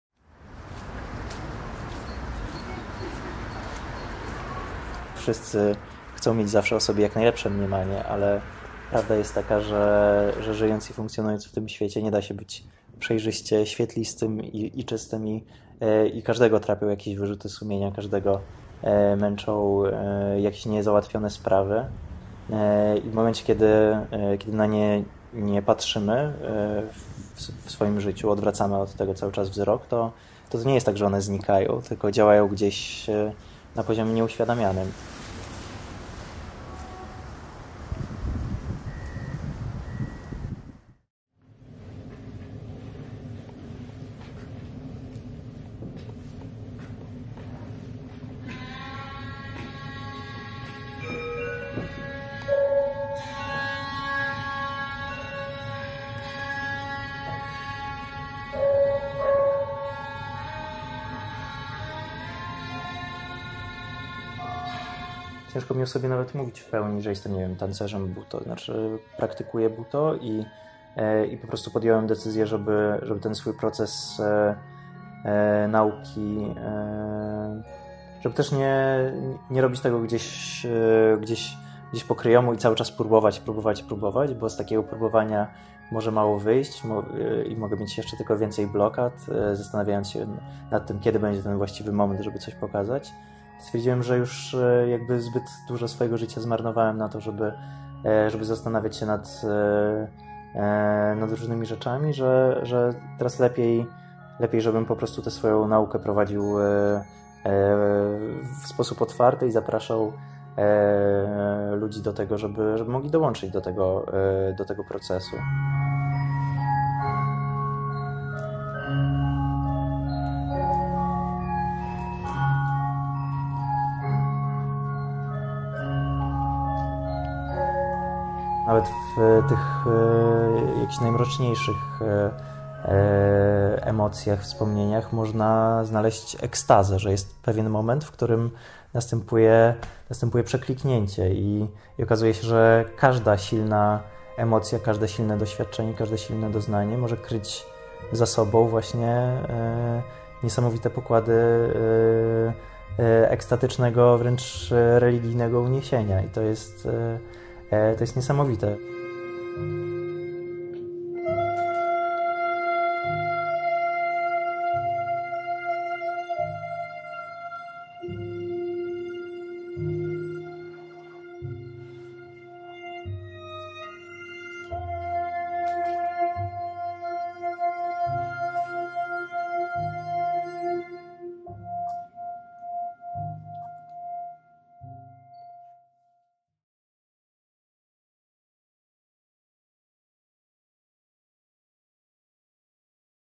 Taniec Mroku
Taniec mroku, w którym odłączasz się od tu i teraz, po to, by skonfrontować się z głęboko siedzącymi emocjami i traumami. Wojtek tańczył Butoh podczas każdego nowiu i pełni księżyca przez cały ostatni rok. Księżycowy Cykl Butoh zrealizował w Polsce, we Włoszech, w Danii i na Teneryfie.